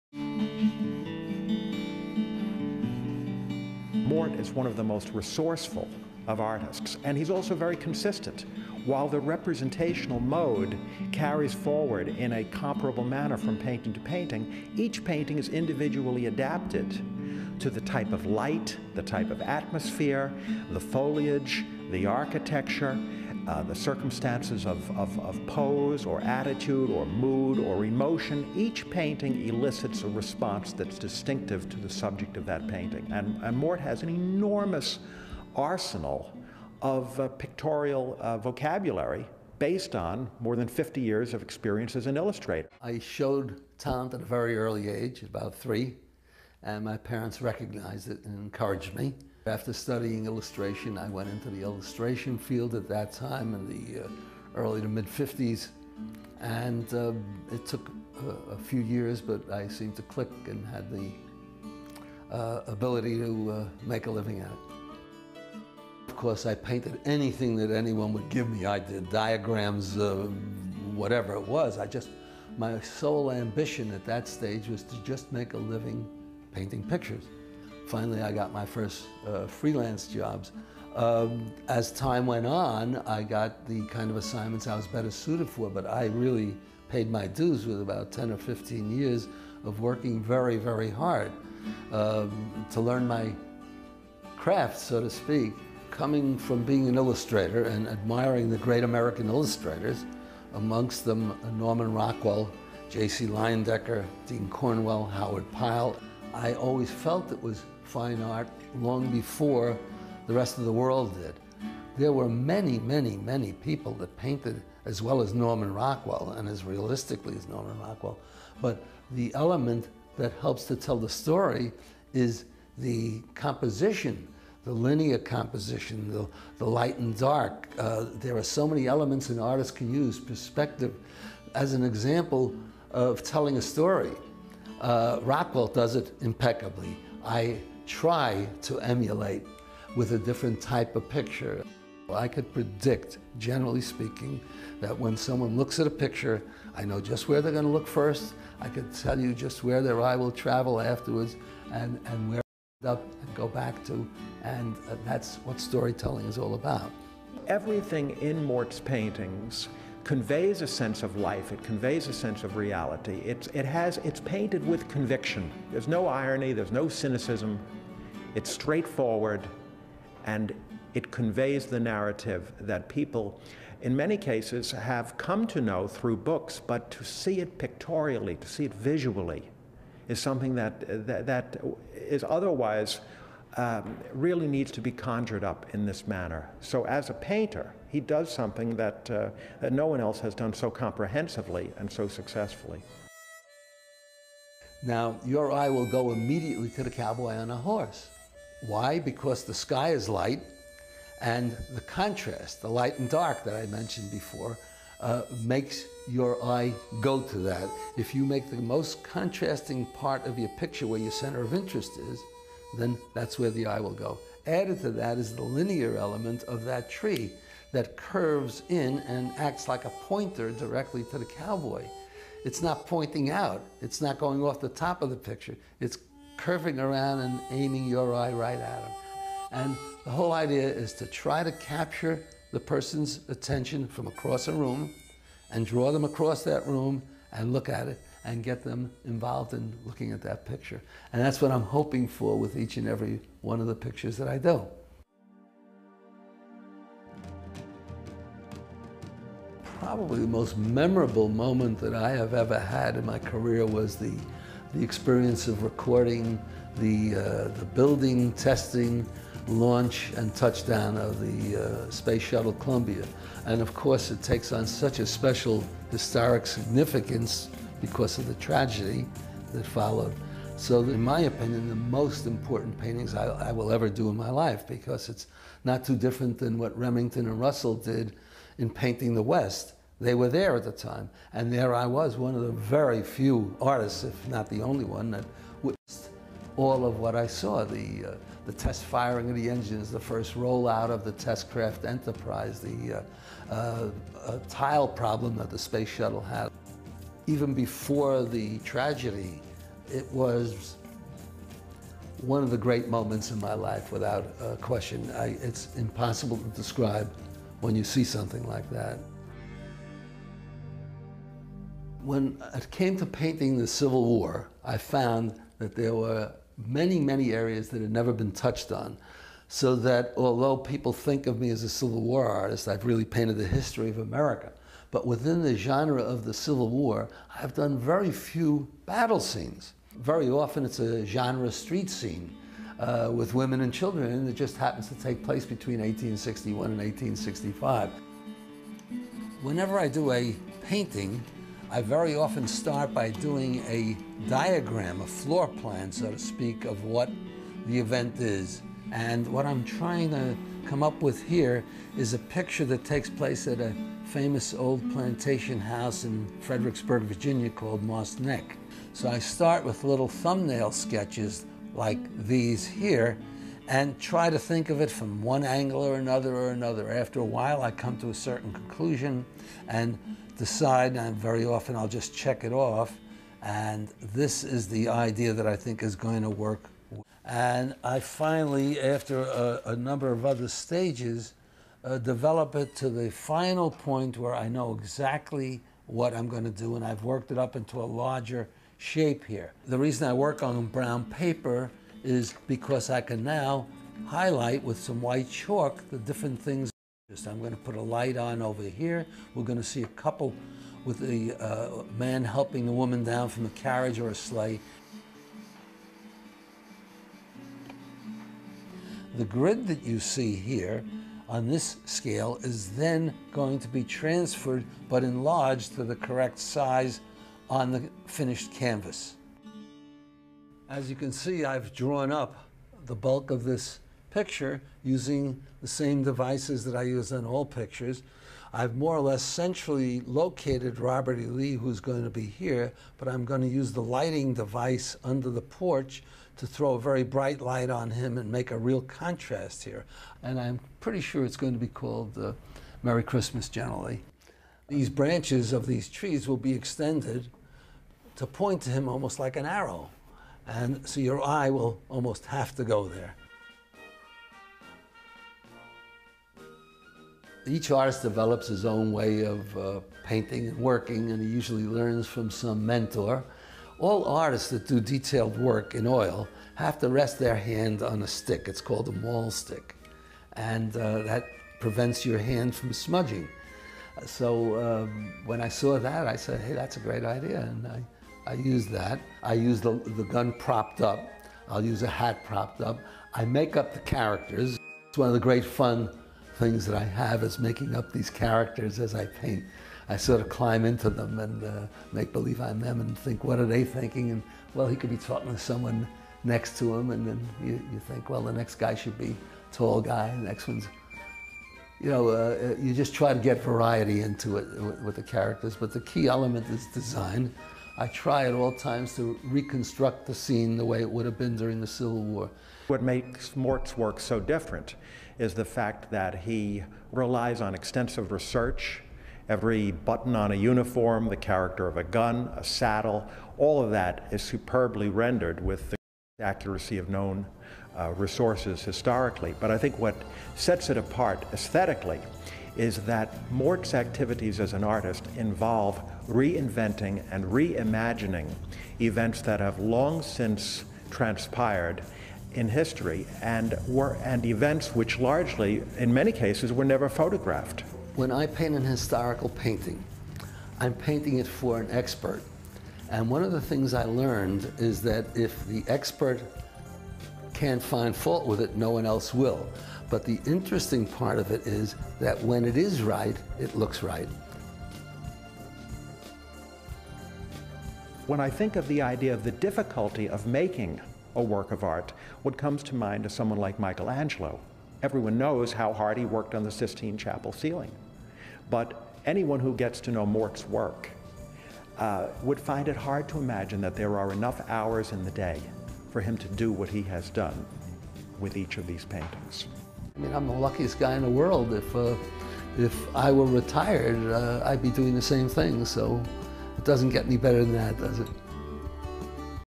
Mort Künstler Exhibit at the Nassau County Museum of Art
America's Most Collected Civil War Artist talks about his career and painting process. Includes footage from "The American Spirit" exhibit.

Filmed: 2006

Filmed in conjunction with the exhibition "The American Spirit – The Paintings of Mort Künstler" at the Nassau County Museum of Art in Roslyn, New York – August 27 – November 5, 2006.

About the Artist:
Mort Künstler is renowned as “the premier historical artist in America.”  When he began his emphasis on Civil War art in the late 1980s, he had already accomplished more than a half-dozen artists could hope to accomplish in a collective lifetime. From portraits of prehistoric American life to the odyssey of the space shuttle, Mort Künstler had painted America's history.

When he placed his focus on Civil War art, Mr. Künstler quickly established himself as the country's most-collected Civil War artist, and earned unprecedented acclaim within the genre of Civil War art.

Mr. Künstler's talent and training were nurtured from an early age. After studying art at Brooklyn College, U.C.L.A. and Pratt Institute, he became a successful illustrator in New York where he received assignments from book and magazine publishers. It was through their assignments of historical subject matter that he learned the value of working with historians so that accuracy was firmly imbued into his concept.